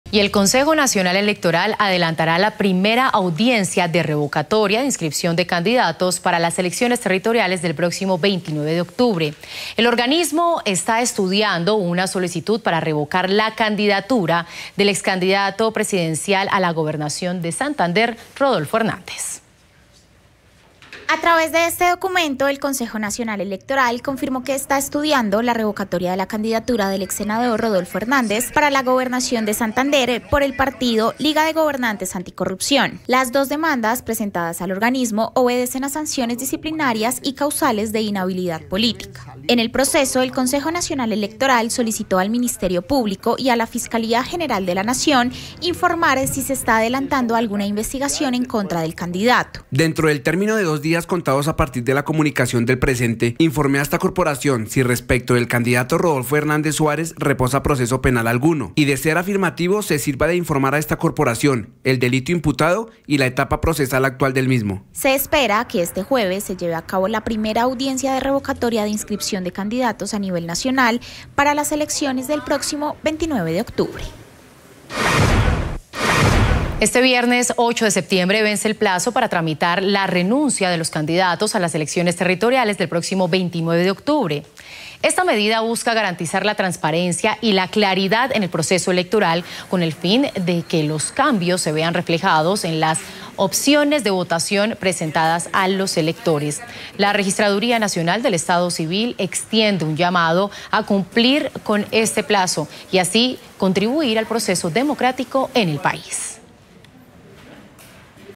Estudian revocatoria de candidatura de Rodolfo Hernández
⭕ 🗳 | El Consejo Nacional Electoral indicó que está estudiando la revocatoria de la candidatura del exsenador Rodolfo Hernández a la Gobernación de Santander.